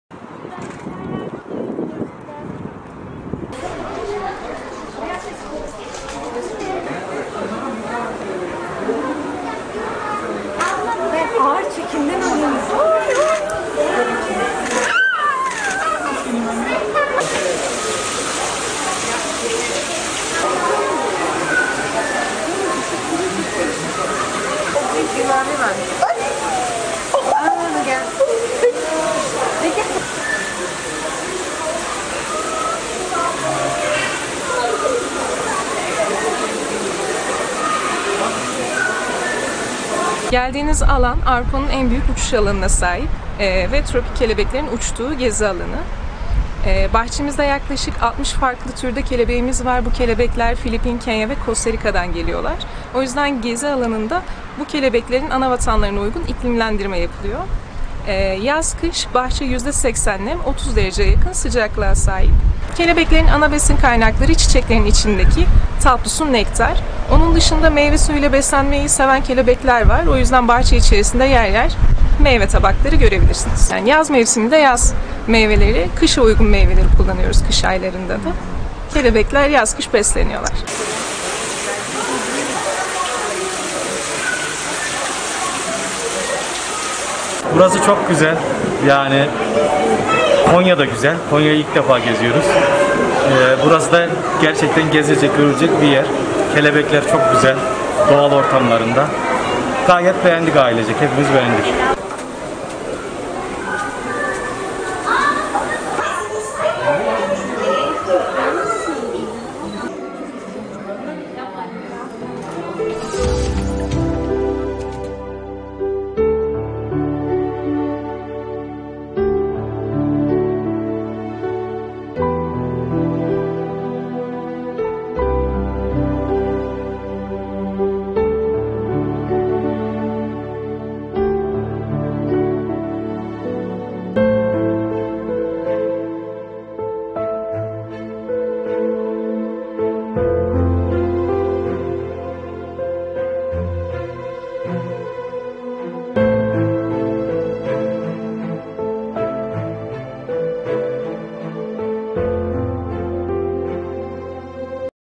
Kelebek bahçesinin narin sakinleri, mevsim meyveleriyle besleniyor
Konya'da beslenme, iklimlendirme ve bitki varlığıyla özel koşullar sağlayan Kelebek Bahçesi, tropikal ülkelerden gelen kelebeklere doğal yaşam alanını aratmıyor.